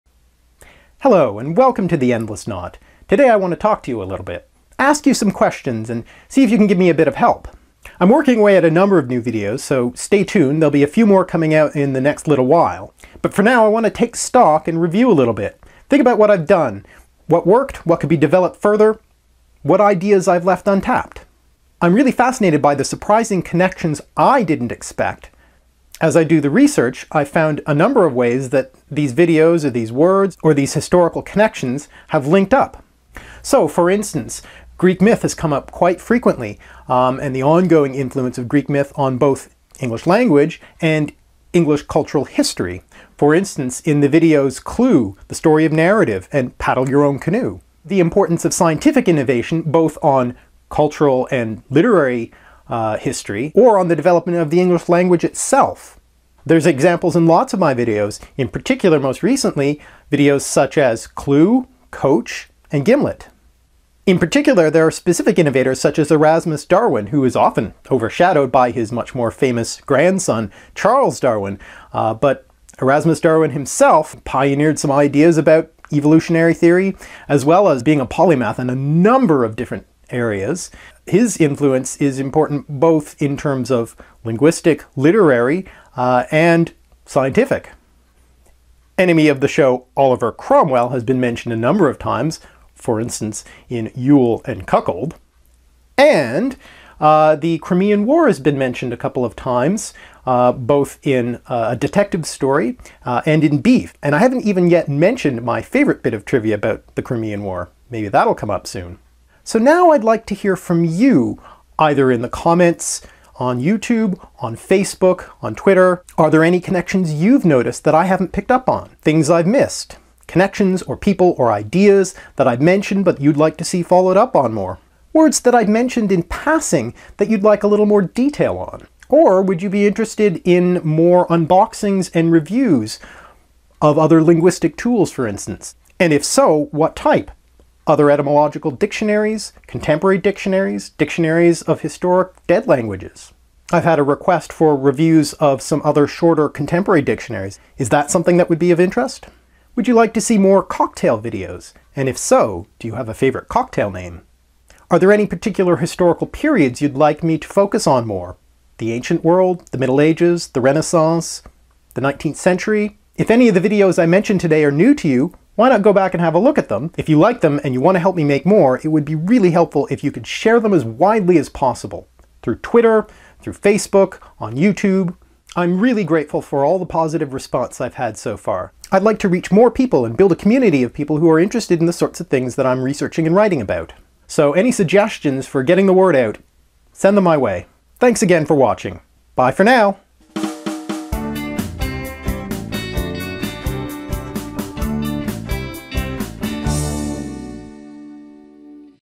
Asking for feedback
Time to take a moment to look at how things are going, and where they should go in the future. Asking for your help -- and thanking you for watching!